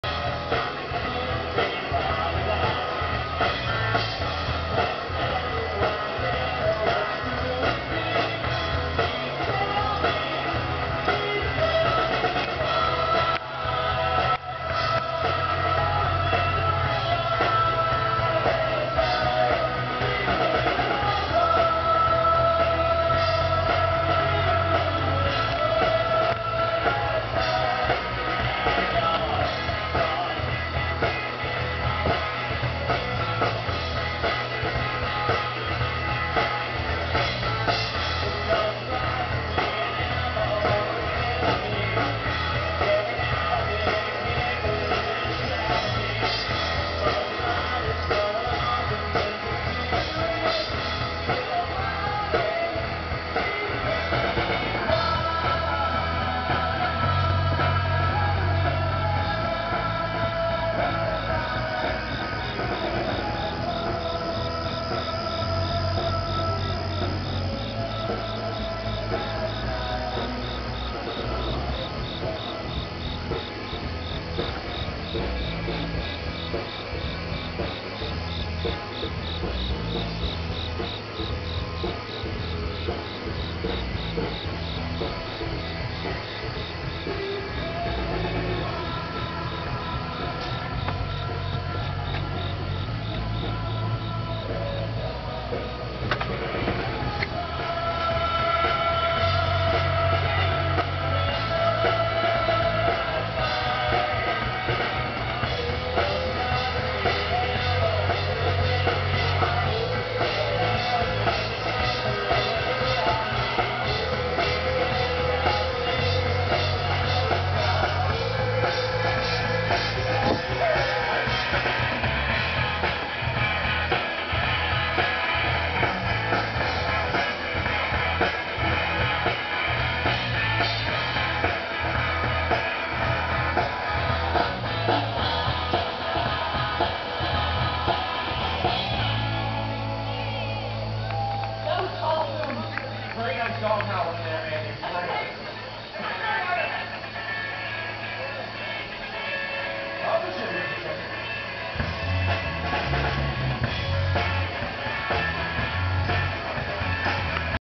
"Jack The Penguin" at the Kitty Knight House
On the boat at Georgetown Yacht Basin Labor Day Weekend '09.